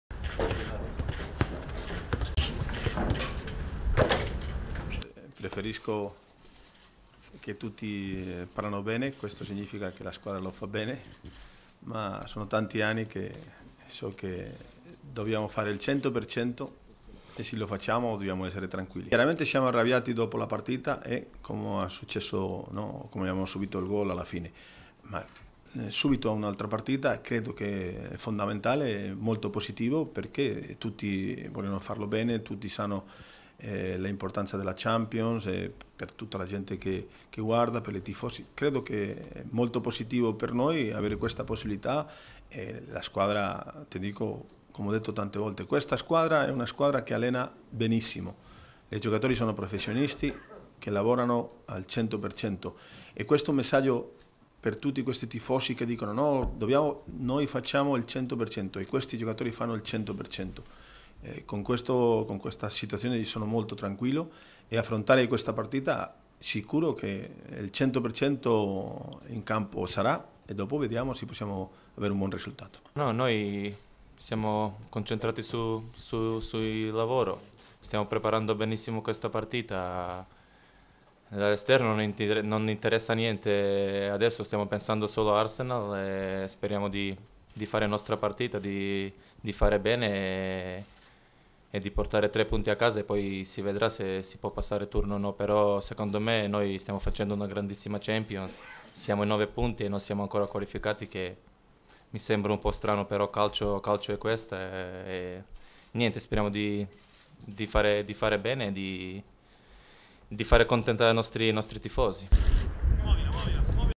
Napoli - Benitez e Pandev: "Daremo tutto contro l'Arsenal" -1- (10.12.13)
- Napoli. Ecco le parole del tecnico alla vigilia del big match di Champions League che deciderà la permanenza degli azzurri nell'Europa che conta. (10.12.13)